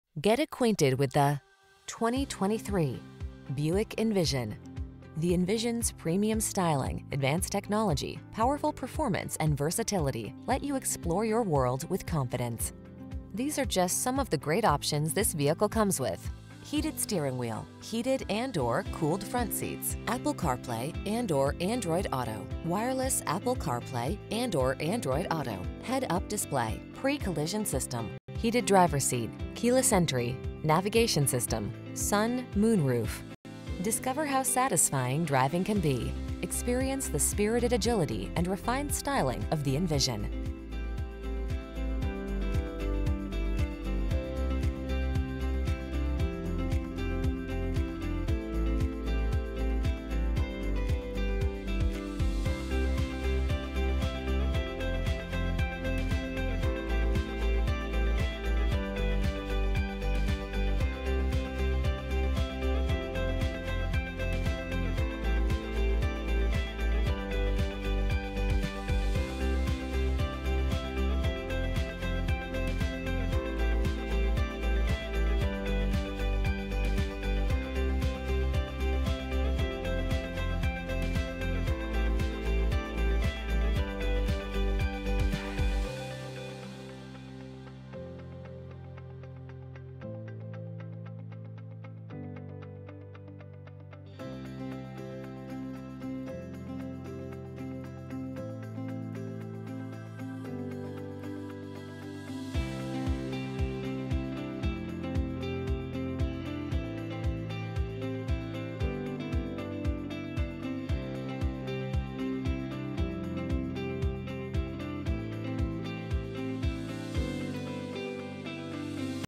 2023 Buick Envision Troy, Dayton, Tipp City, Piqua, Casstown B16991
Cinnabar Metallic New 2023 Buick Envision available in 3540 S. Co Rd 25A, Troy, Ohio at Dave Arbogast Buick GMC. Servicing the Troy, Dayton, Tipp City, Piqua, Casstown, area.

3540 S Co Rd 25A

Recent Arrival! $1440 off MSRP! 22/29 City/Highway MPGbrbr2023 Buick Envision Avenir 2.0L TurbochargedbrbrABS brakes Adaptive Cruise Control Alloy wheels Compass Electronic Stability Control Front dual zone A/C Heated Driver & Front Passenger Seats Heated front seats Heated rear seats Illuminated entry Low tire pressure warning Navigation System Power Heated door mirrors Power Liftgate Power Sunroof/Moonroof Remote keyless entry Traction control.brbrbr*See Dealer For Specific Equipment* Dealer Disclosure: Sales Tax Title License Fee Registration Fee $250 Dealer Documentary Fee are additional to the advertised price.